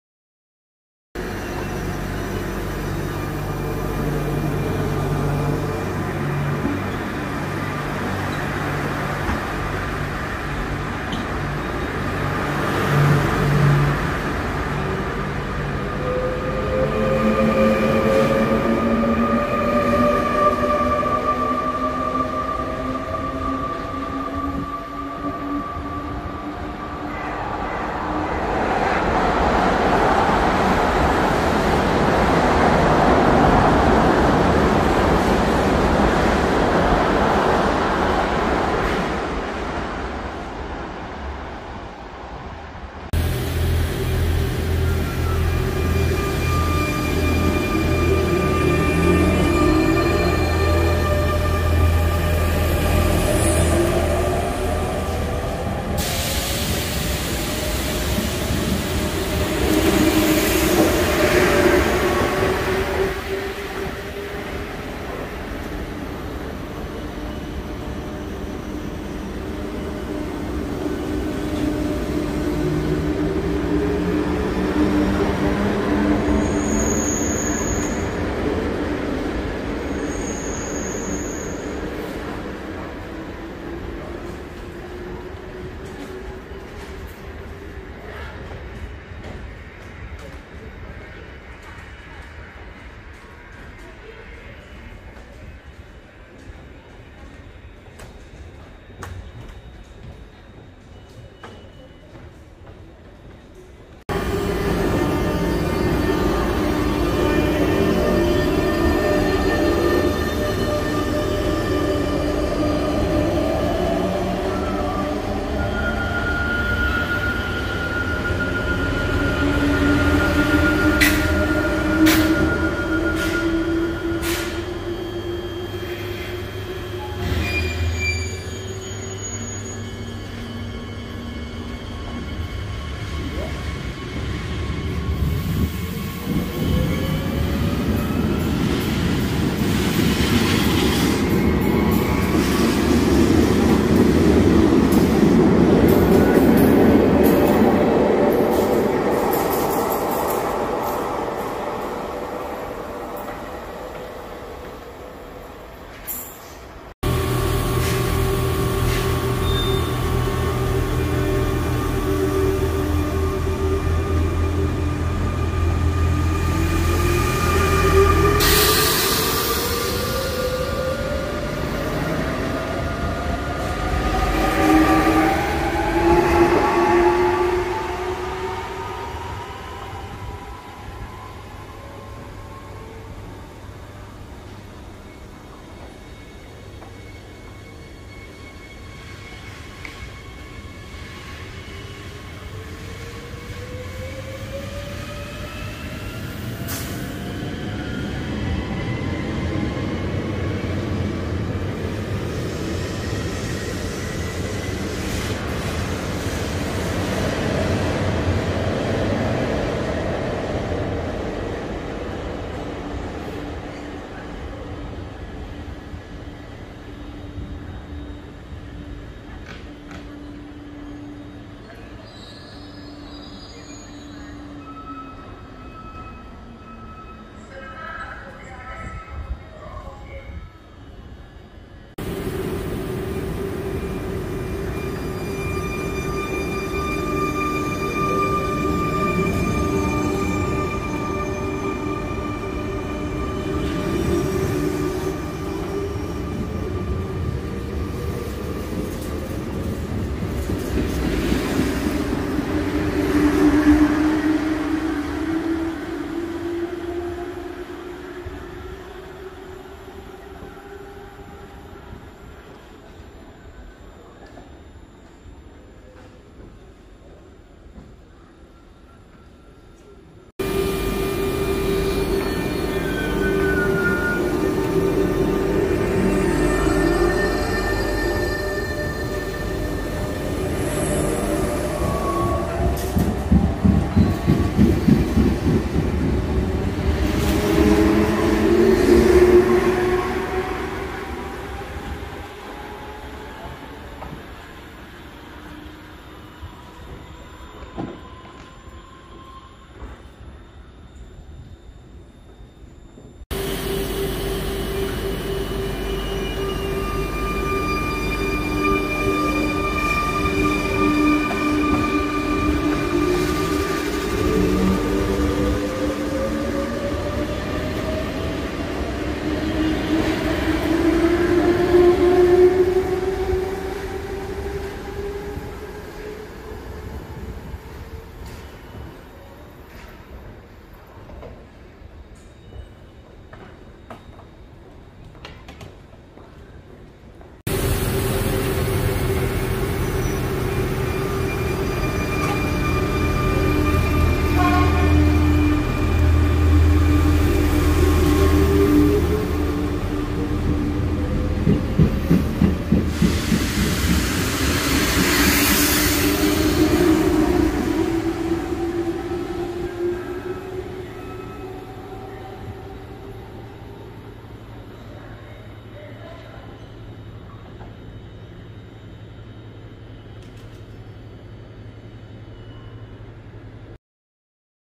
[RER C] [Transilien N] Compilation de Z 2N/Z 57000 en gare de Viroflay-Rive-Gauche
Éléments : 19B et 217A
                     ...R et ...R
                     208A et 186A
                     33T
                     ...R et ...R
                     39B et 37A
                     ...R et 118R
                     22B et 243A
                     127R et 120R
                     113R et ...R
                     246A et 23B
                     18C et 157A
                     248A et 56B
                     217A et 19B
                     118R et ...R🙂

Vidéo disponible jusqu'en 4K 60ips pour les appareils adaptés😉

Copie ou utilisation de la vidéo interdite sans mon autorisation